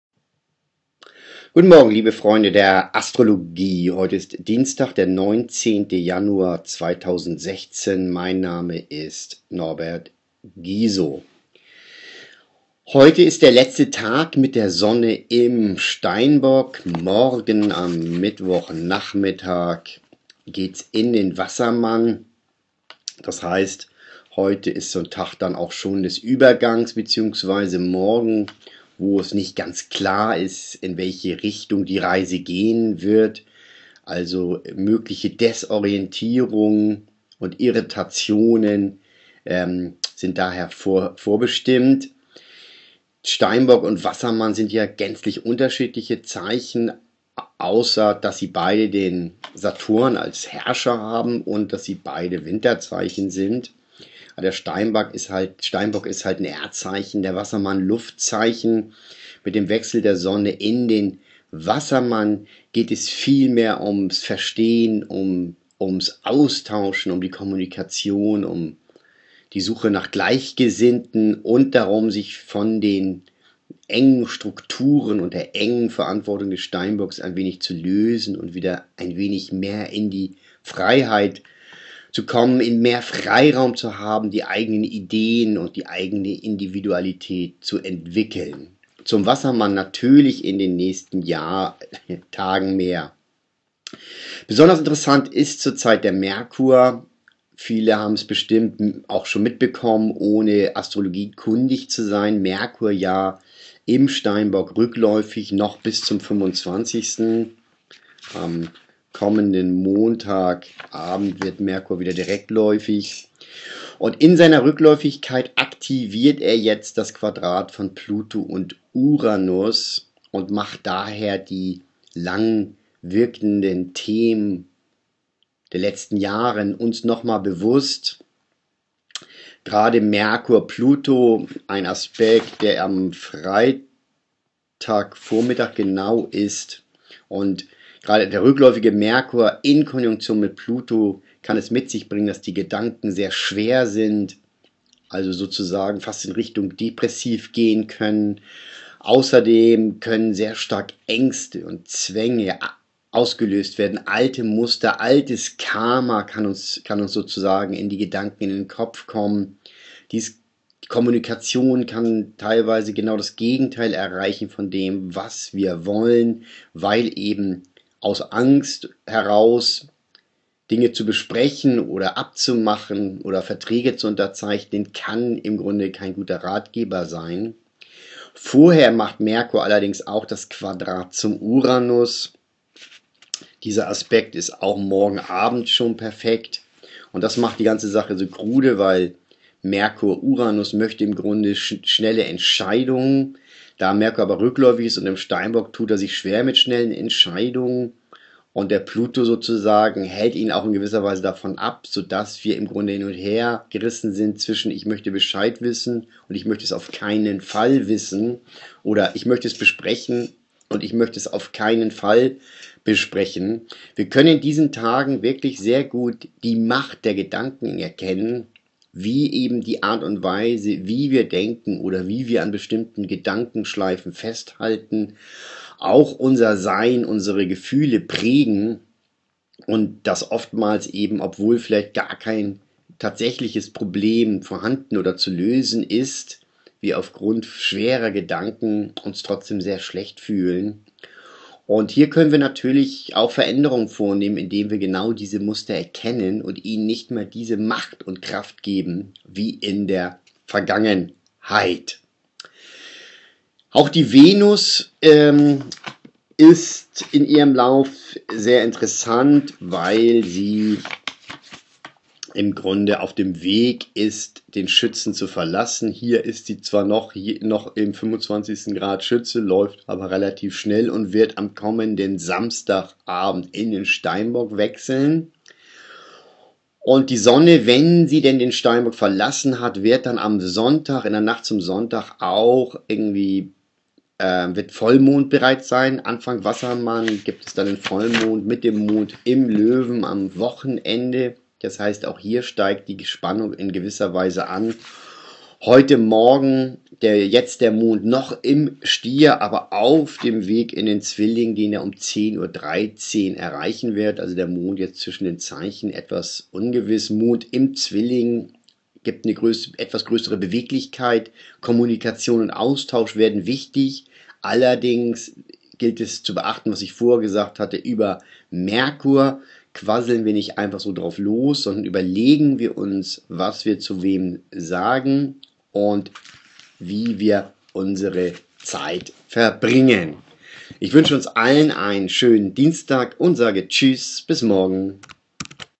Tageshoroskop Dienstag 19. Januar 2016
und präsentieren das Tageshoroskop vom 19. Uns kann in diesen Tagen die Macht der Gedanken sehr deutlich werden. Morgen wechselt die Sonne in den Wassermann, am Sonntag ist Vollmond. Mehr im Video...